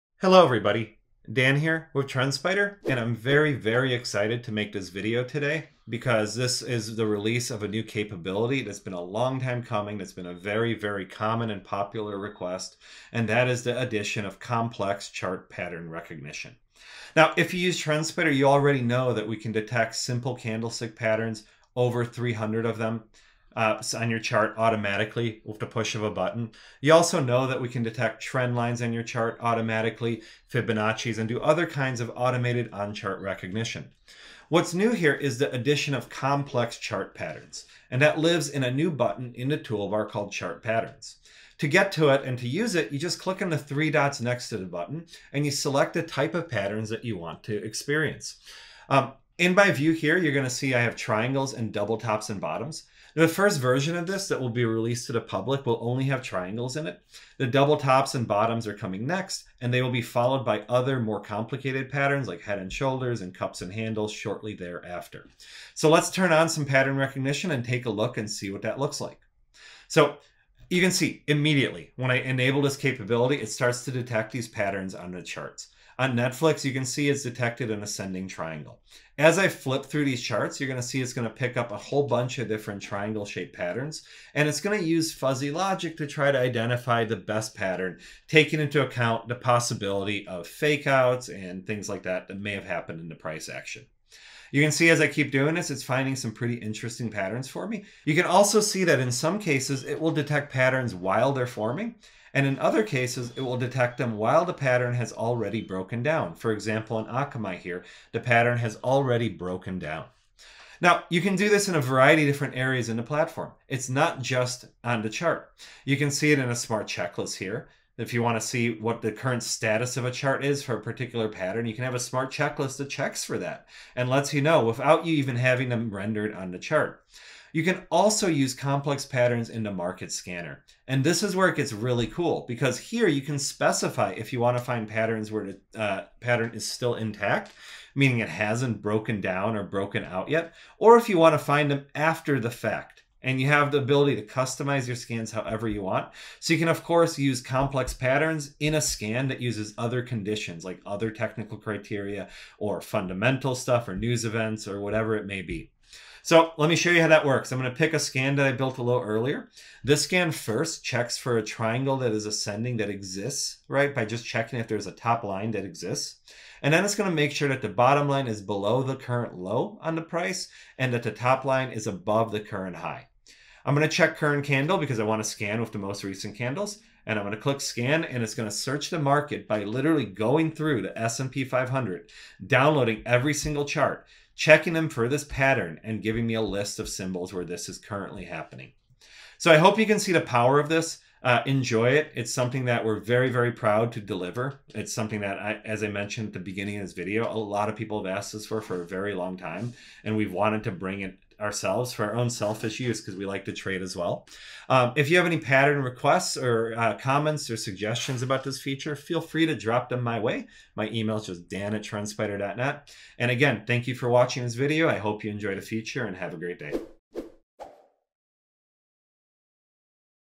New Feature! Introducing Automated Chart Pattern Recognition
⚡️New Feature! Automated Chart Pattern Recognition is now available on TrendSpider!

In this video learn how to:
✅ Add chart patterns
✅ Use them across the platform
✅ And more!

00:00 Introducing Automated Pattern Recognition
00:42 How to Add Automated Patterns to Your Chart
01:52 Additional Capabilities
02:40 How to Use Automated Pattern Recognition